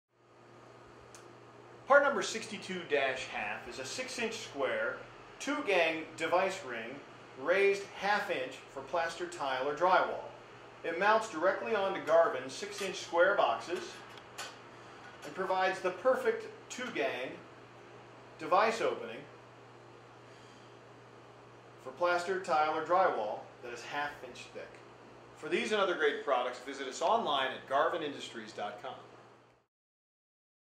Square Box Cover: 1/2" Raised 6" Square Two Gang Device Covers 62-1/2
- 1/2" raised device covers are used to mount numerous electronic and electrical devices to 6" square boxes. Garvin Industries' two gang device covers quickly and securely mount to boxes and provide access to install switches, devices, receptacles and other electronic/electrical appliances.